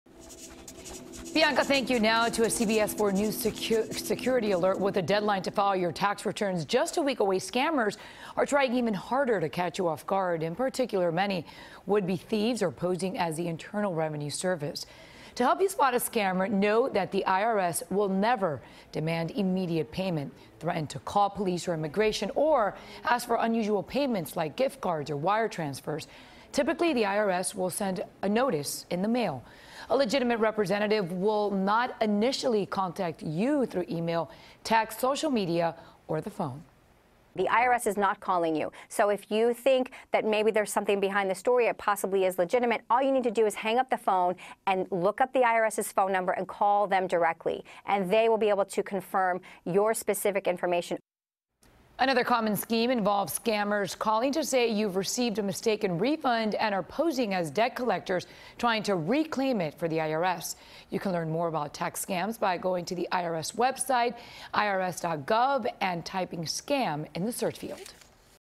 IRS Warning Taxpayers About Scams
IRS will never call you or demand immediate payment